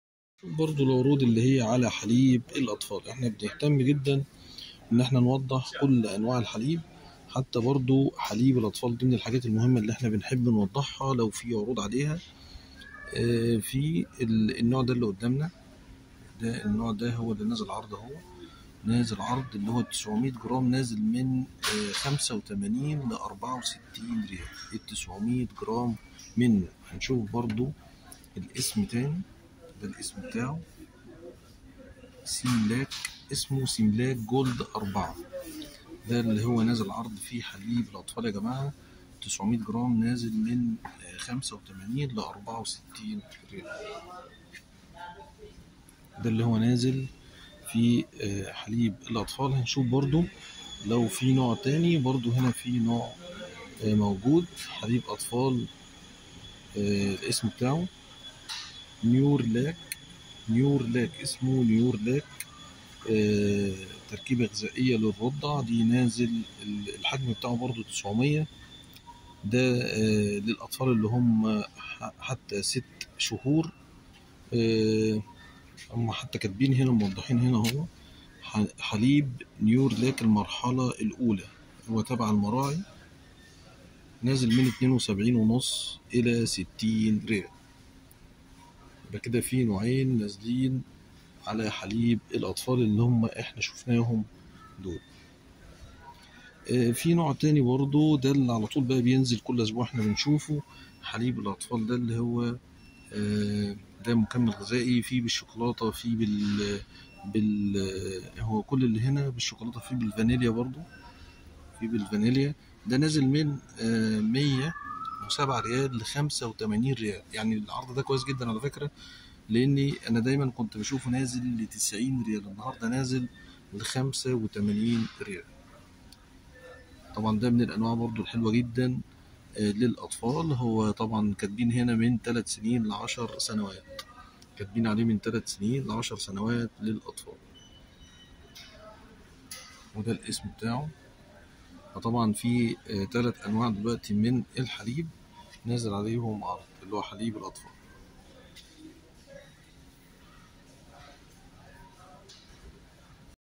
عروض حليب الأطفال في أسواق بندة عروض تنتهي الثلاثاء ٧ فبراير لو محتاجها باقي يومين
يسعدنا جدا تواصلكم معنا في الاستفسار عن اي شيء